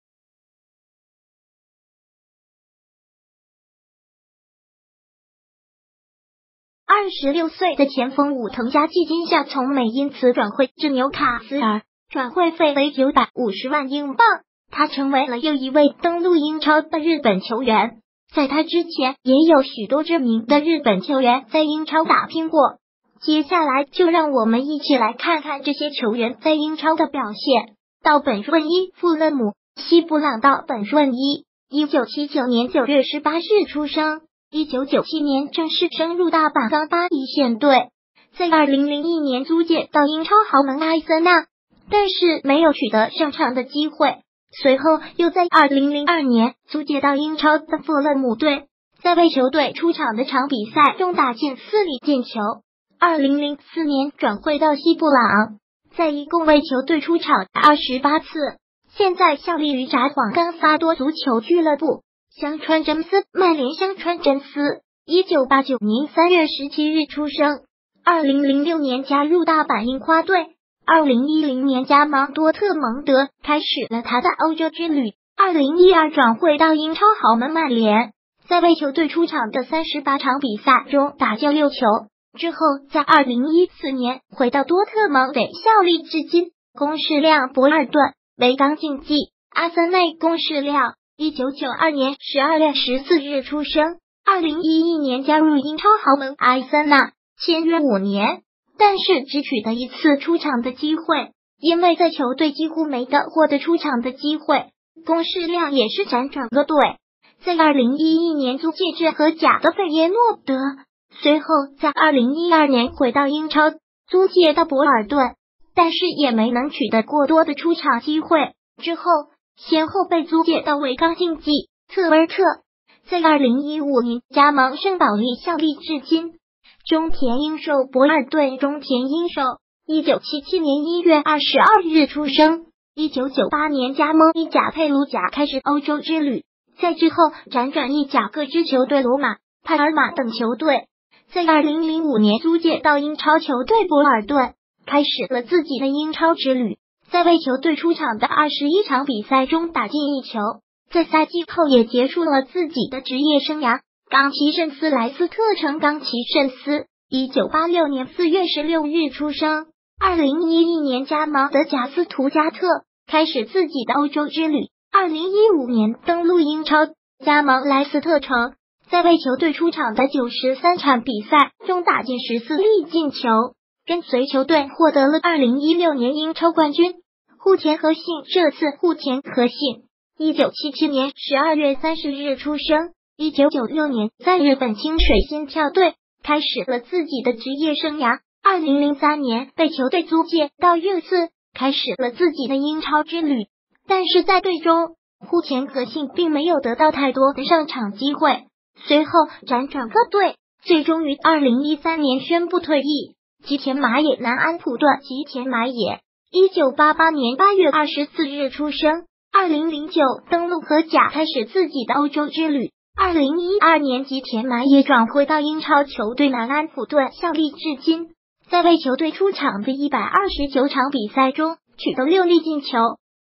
在英超效力的日本球员，都过得怎么样？
26岁的前锋武藤嘉纪今夏从美因茨转会至纽卡斯尔，转会费为950万英镑，他成为了又一位登陆英超的日本球员。在他之前也有许多知名的日本球员在英超打拼过，接下来就让我们一起来看看这些球员在英超的表现。稻本润一(富勒姆、西布朗)稻本润一，1979年9月18日出生，1997年正式升入大阪钢巴一线队。在2001年租借到英超豪门阿森纳，但是没有取得上场的机会。随后又在2002年租借到英超的富勒姆队，在为球队出场的场比赛中打进4粒进球。2004年转会到西布朗，在一共为球队出场28次。现在效力于札幌冈萨多足球俱乐部。香川真司(曼联)香川真司，1989年3月17日出生，2006年加入大阪樱花队。2010年加盟多特蒙德开始了他的欧洲之旅。2012转会到英超豪门曼联，在为球队出场的38场比赛中打进6球。之后在2014年回到多特蒙德效力至今。宫市亮(博尔顿、维冈竞技、阿森纳)宫市亮，1992年12月14日出生，2011年加入英超豪门阿森纳，签约5年，但是只取得1次出场的机会。因为在球队几乎没得获得出场的机会，宫市亮也是辗转各队，在2011年租借至荷甲的费耶诺德，随后在2012年回到英超，租借到博尔顿。但是也没能